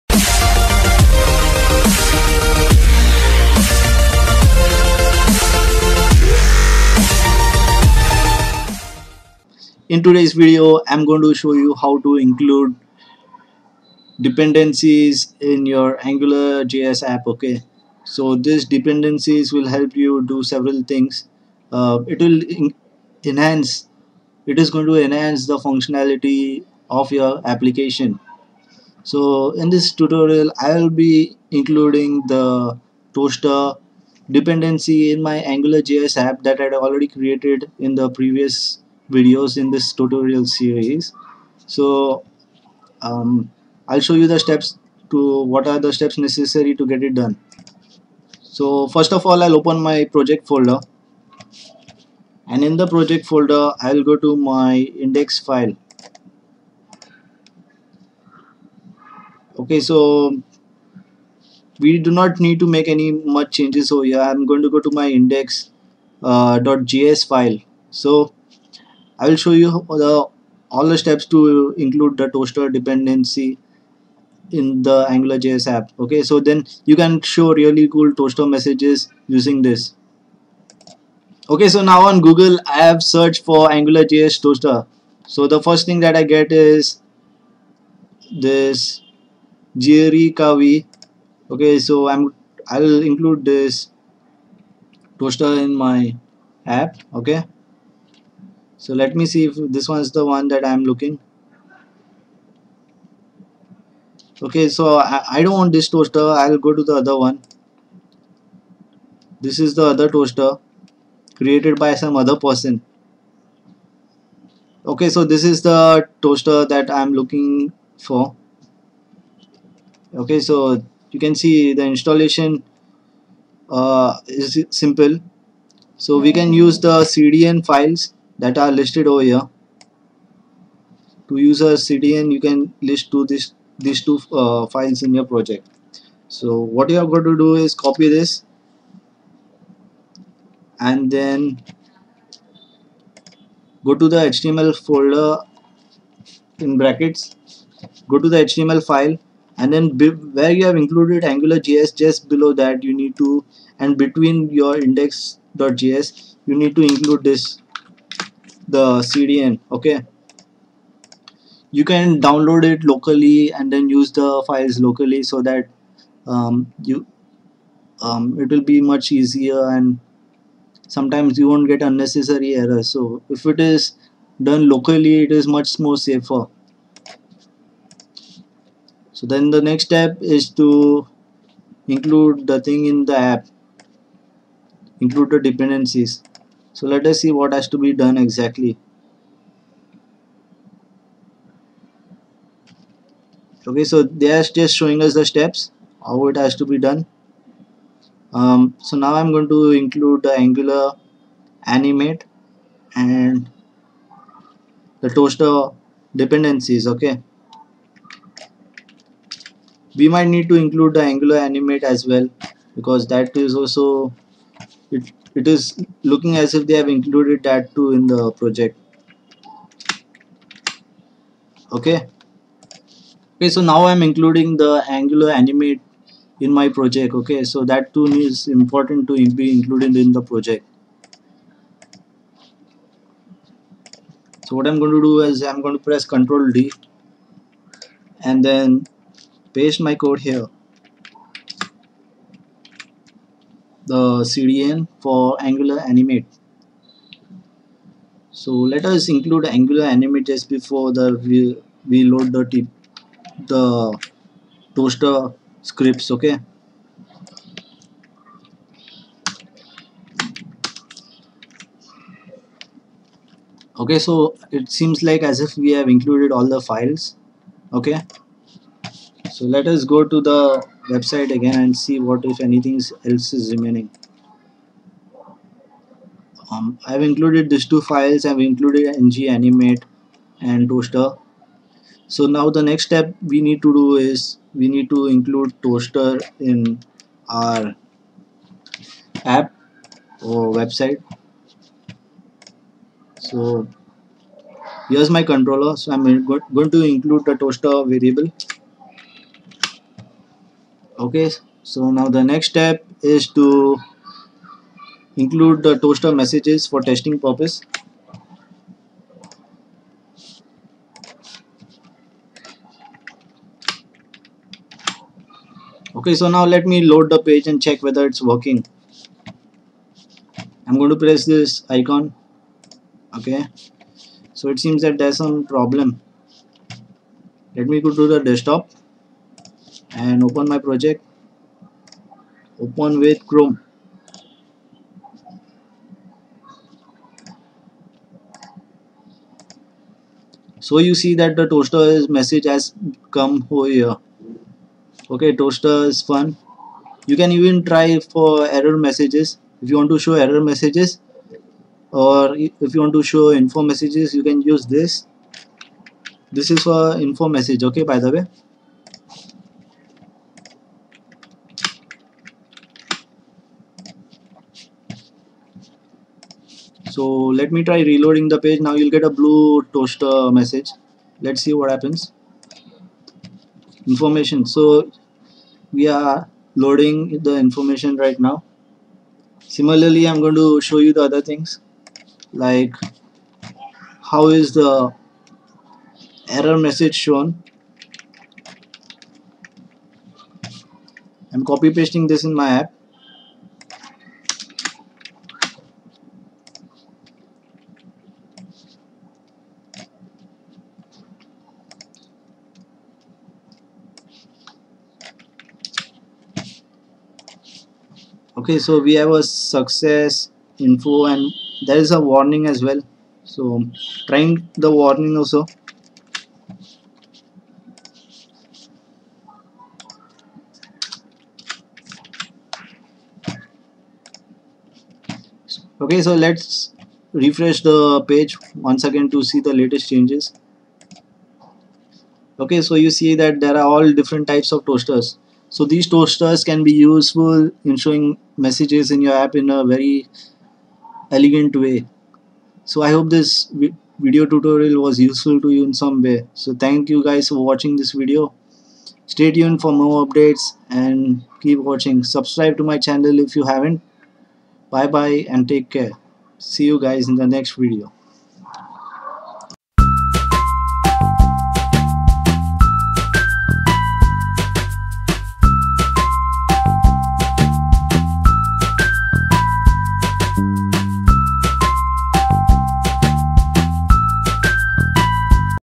#5 Angular & Javascript Series-Angular Dependancies and Angular Toaster
Buy Reliable Web Hosting for your Website & Apps from bluehost

In this Video I show about Dependencies in angular js using Toaster